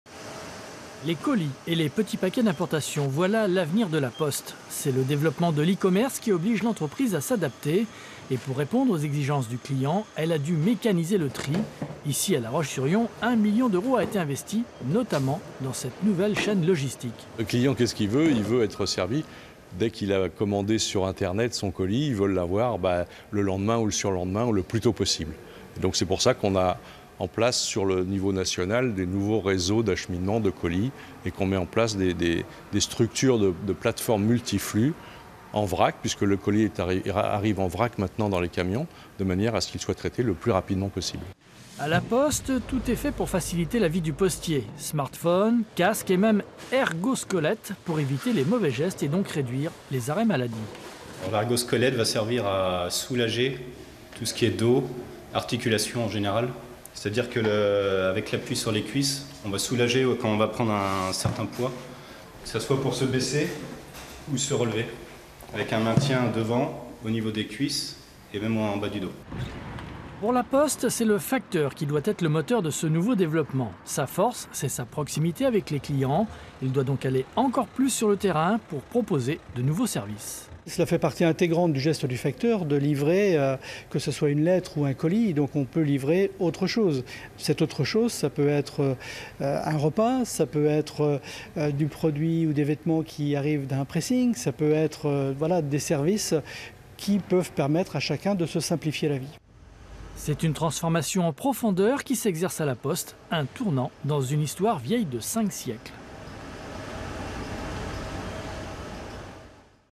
Vendée : moins de courrier, la Poste s'adapte
Le volume de courrier a été divisé par deux en 10 ans. Mais avec l'essor du commerce sur Internet, les colis eux, sont de plus en plus nombreux. La Poste adapte ses outils.

►Reportage de Thierry BERCAULT et Boris VIOCHE

Les personnes interviewées :

THIERRY GARINO (Directeur opérationnel en Vendée),
STEPHANE SAOUDIN (Responsable opérationnel colis 85),
ALAIN CHARRIER (Directeur exécutif des services courrier colis en Pays de la Loire)